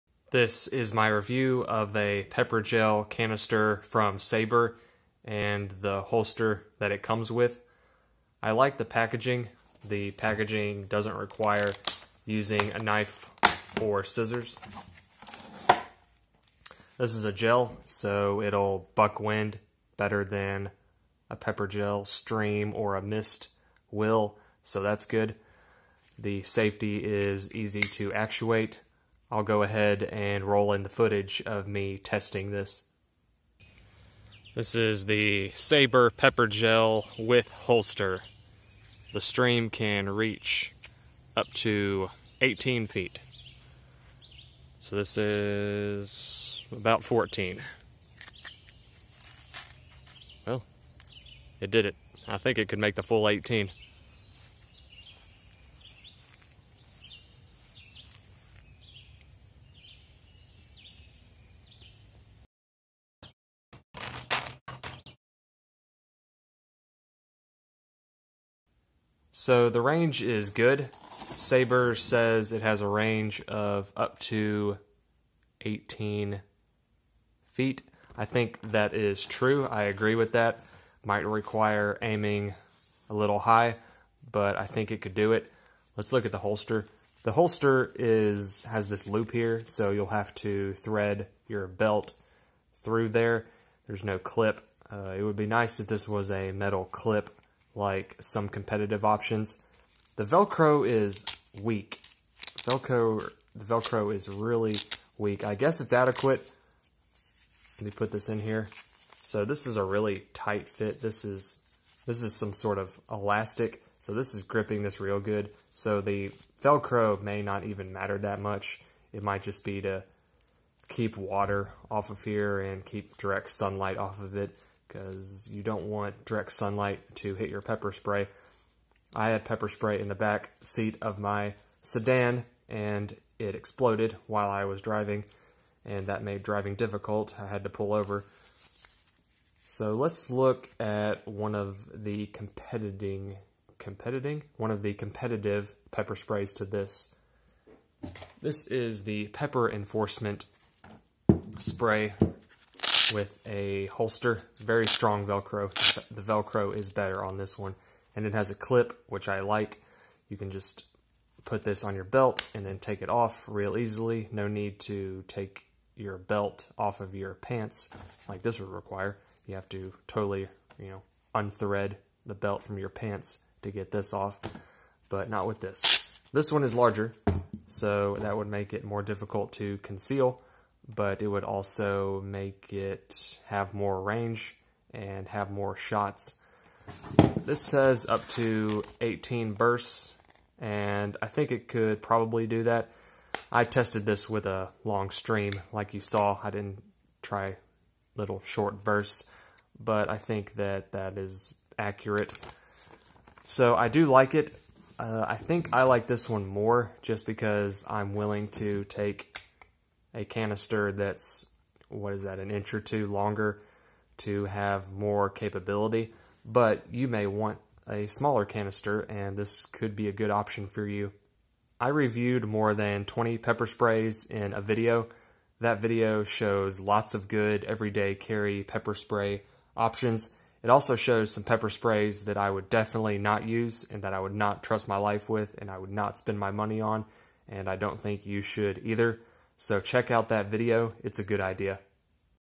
SABRE RED Pepper Gel with Holster - Tested and Reviewed
This is a very good spray. I found from my testing that it will shoot the claimed 18 foot range.

The holster works good. It will hold the can securely. If you want a pepper spray to carry in a belt holster, then this is a very good option.